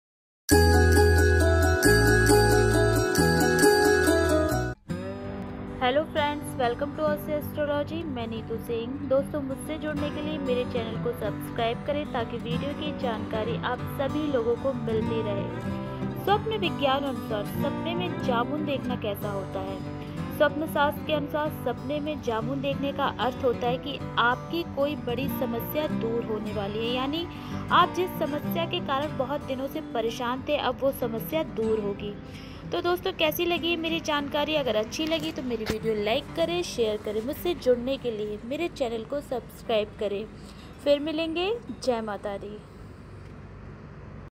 Sapne me jamun dekhna | सपने में जामुन देखना कैसा होता है | Hindi
Dosto, iss video me mene bataya hai ki, sapne me jamun dekhna kaisa hota hai.